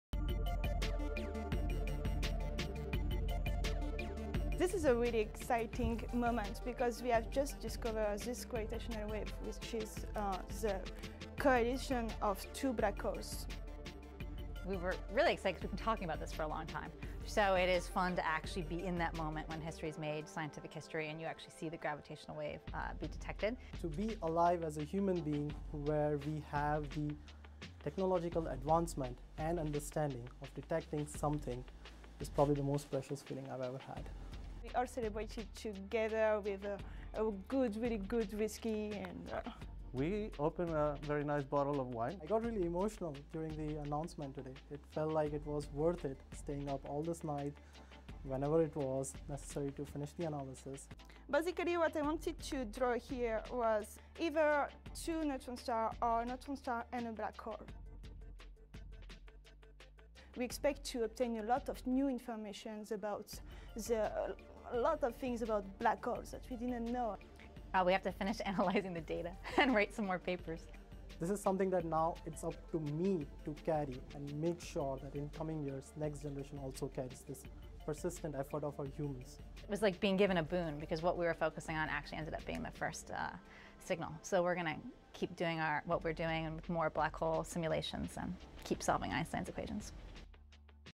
Georgia Tech phd student Karan Jani shares his Gravitational Waves experience
Scientists share their 'gravitational waves' exprience in an interview with CNN news